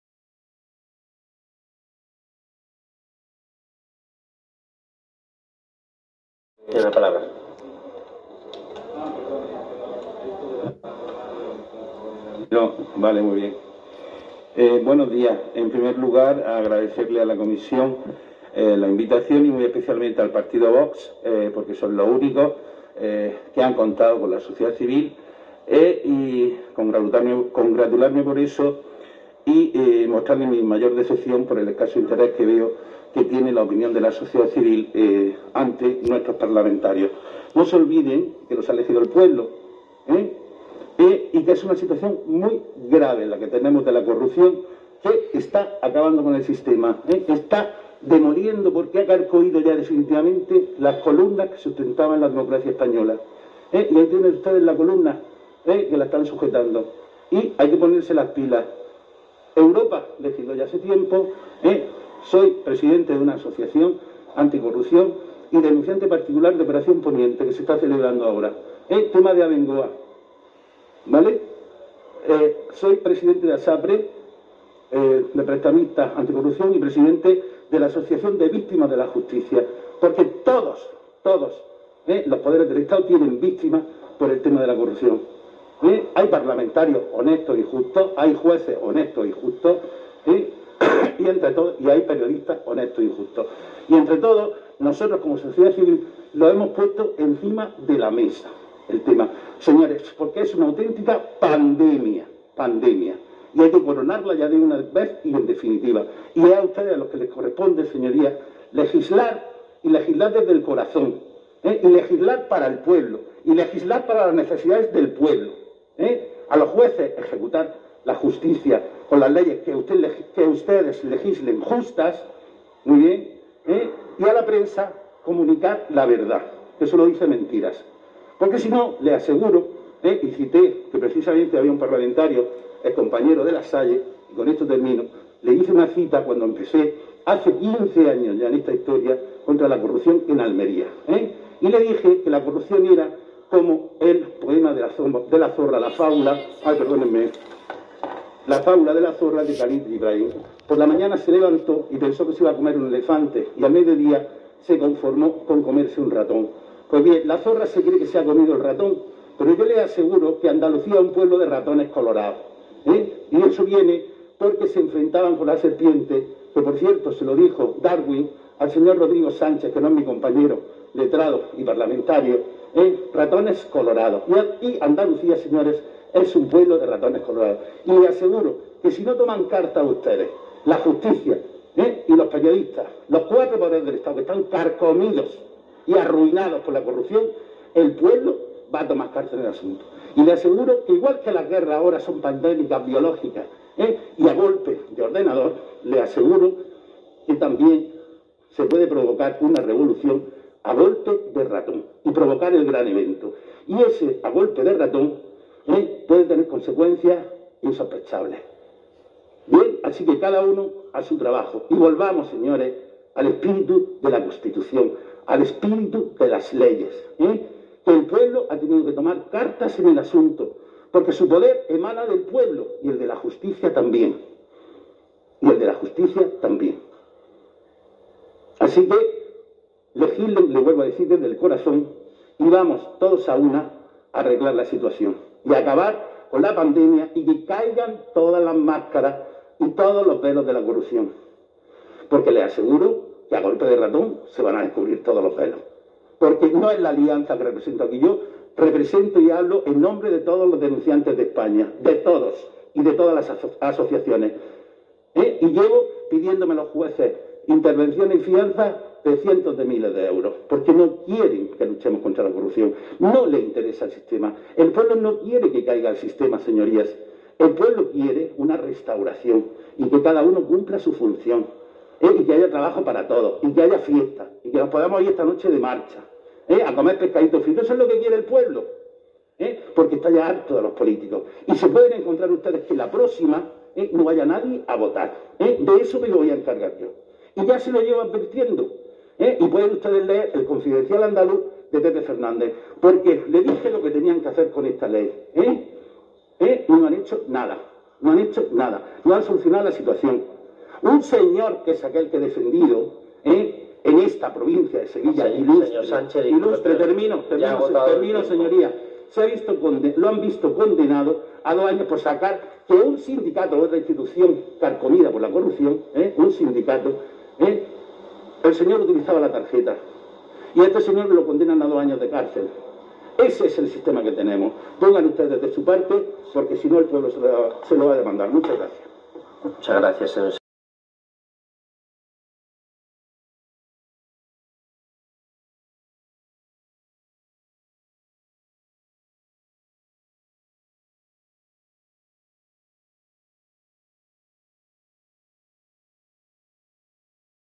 Andalucía Oficina contra el fraude y la corrupción. comparecencia parlamentaria trámite creacion
Asociación Mediterránea Anticorrupción y por la Transparencia